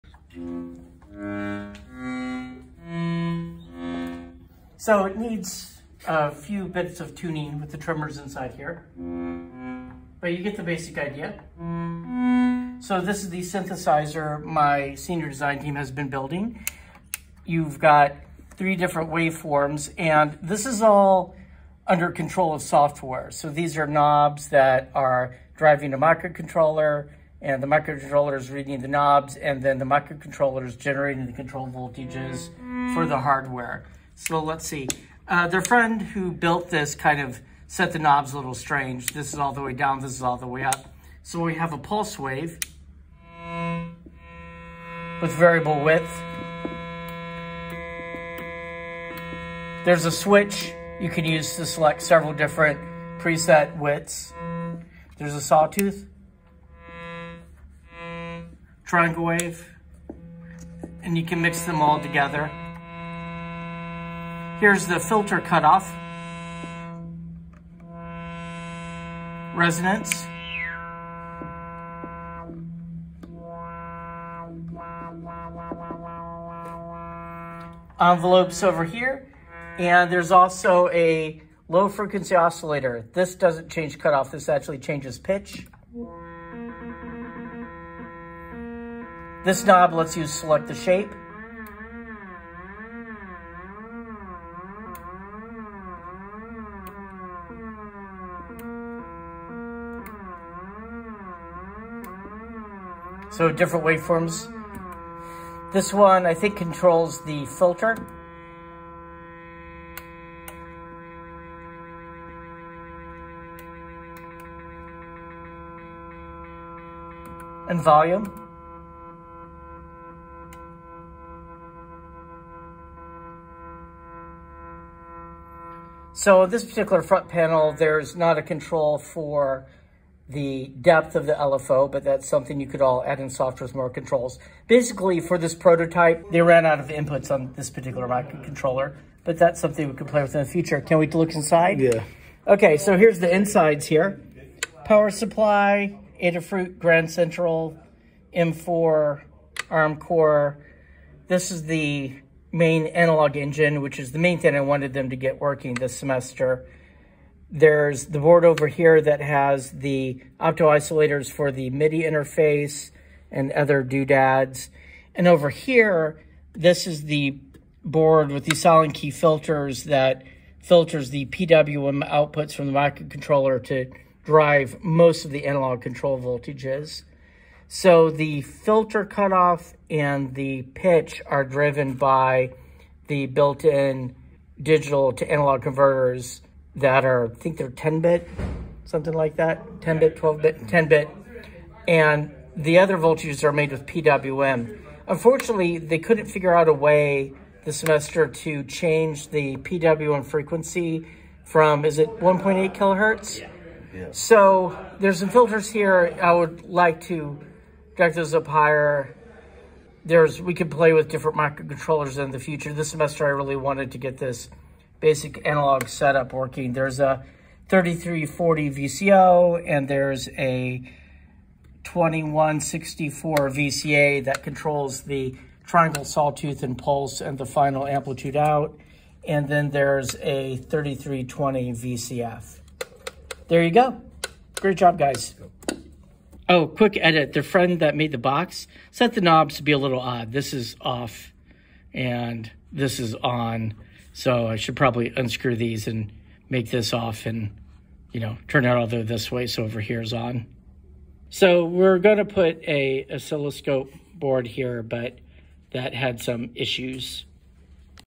Synthesizer Student Project: Analog Signal Path with Adafruit Grand Central M4 Microcontroller
The Adafruit Grand Central M4 has two "real" DACs; these drive the oscillator pitch and VCF frequency. The other analog parameters, such as resonance and amplitudes, are controlled by the Grand Central's PWM outputs (through appropriate filtering). Signal path: 3340 VCO triangle, saw, and pulse outputs, with 3 separate VCAs, one for each output, mixed and sent to a 3320 VCF, then a final VCA. The VCAs are implemented with a 2164.